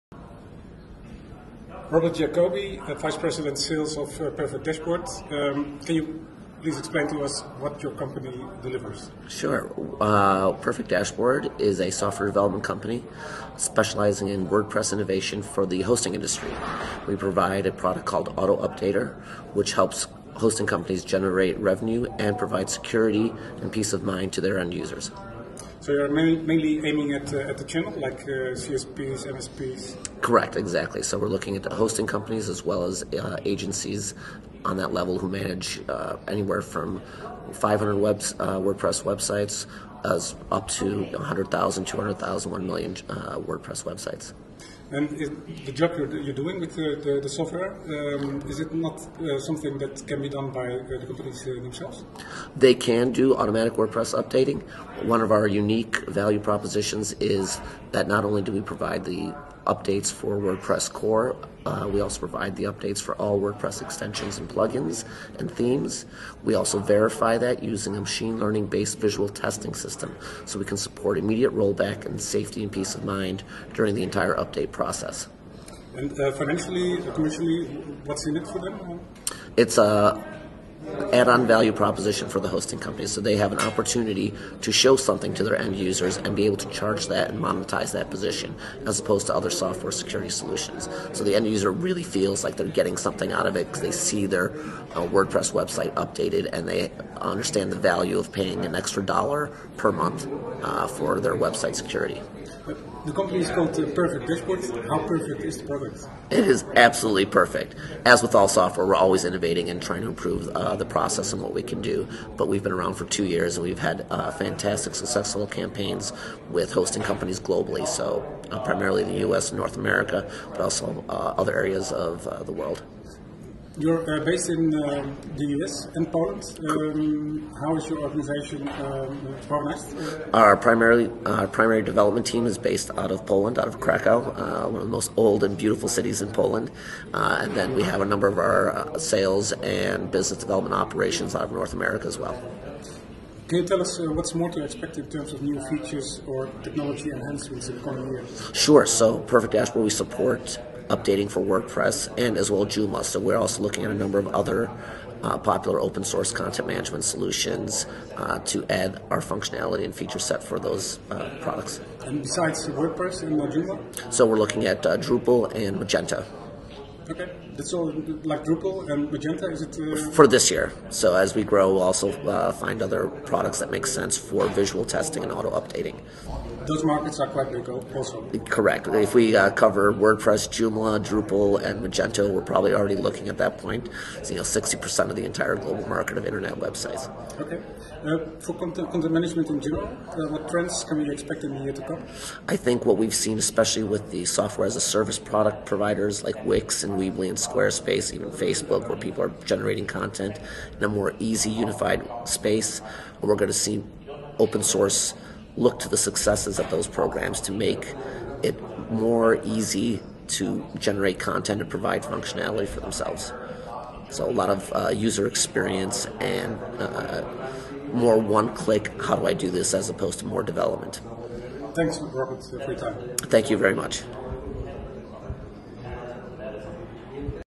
HostingJournalist.com interviews Robert Jacobi (Perfect Dashboard)
Interview recorded at CloudFest 2018 in Rust, Germany.